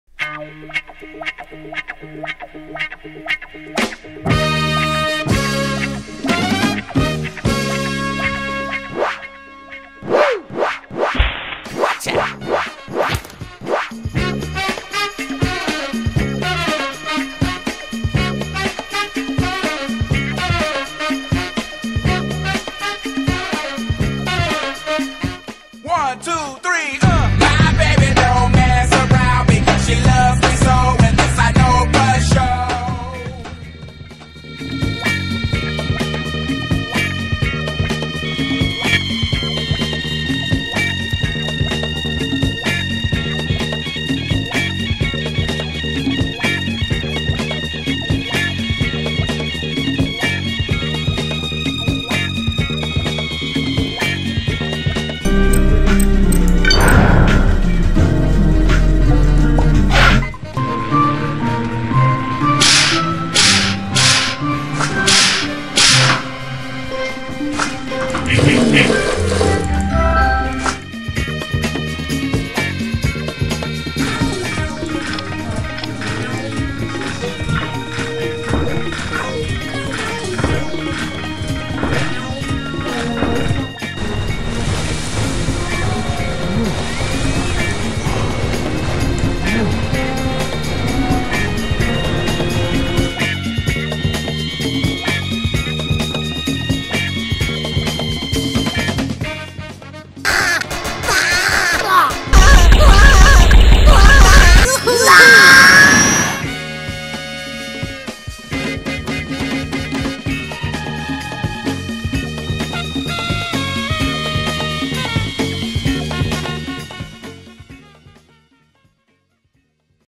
Who is Chugulu Games ? (US version)
Chugulu Games is an agency specialising in game-based interactive communication and advertainment. We conceive and develop digital media products (advergames, social games, serious games).